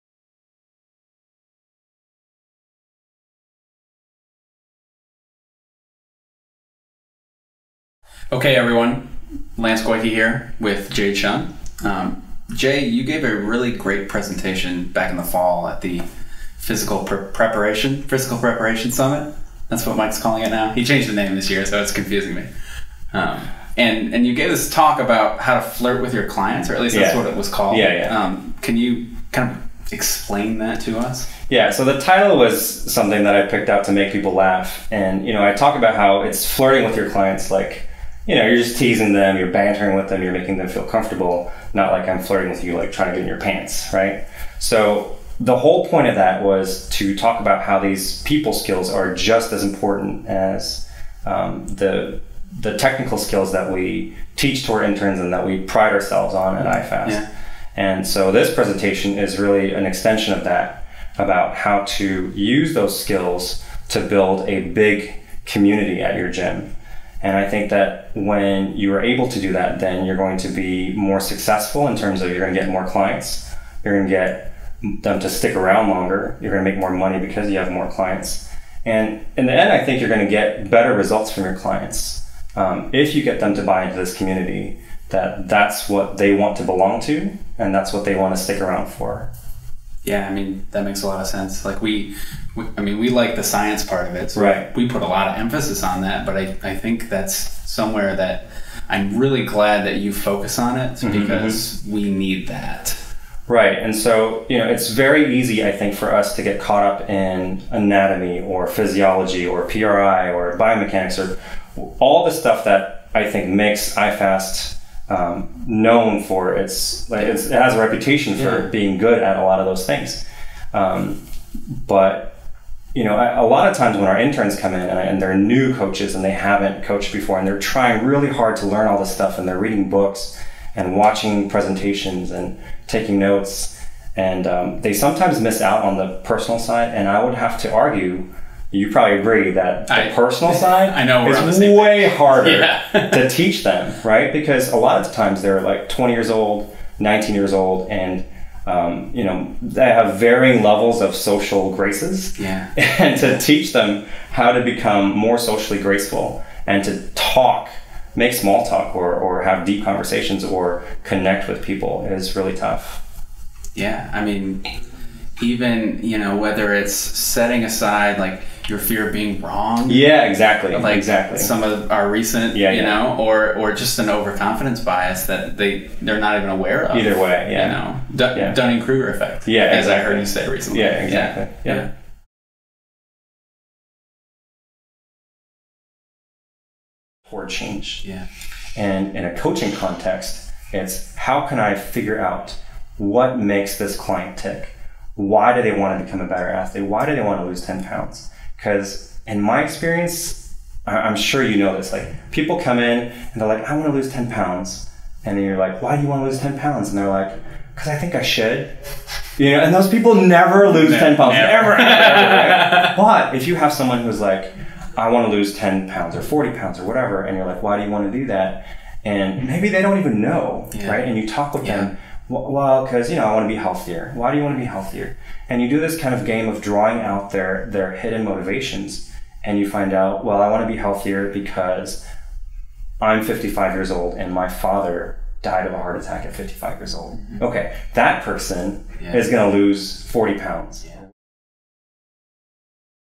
Not Just a Gym: How to build a tribe of fiercely loyal clients
Author: Jae Chung, CSCS, Pn2

How do you turn clients into people who will do anything you ask?

This month's interview with Jae details the first in-service our interns receive. Go use Jae's wisdom to turn regular clients into lifelong residents in your cult of health.

In this video, Jae:
• explains the two snap judgments that people make within seconds of meeting you, and how those judgments can affect your technical and financial success as a coach;
• shows you what traits good leaders, clinicians, and coaches have in common, and how great coaches use these traits to inspire fierce loyalty from their clients and athletes;
• one simple exercise we teach our interns to help them learn how to build better rapport with our clients, especially useful for those who consider themselves to be introverted, shy, or just “not great with people”

Whether you’re the owner of a thriving gym, or dreaming of getting your first client, this presentation will help you identify exactly what kind of community you want your gym to be, and how to get there.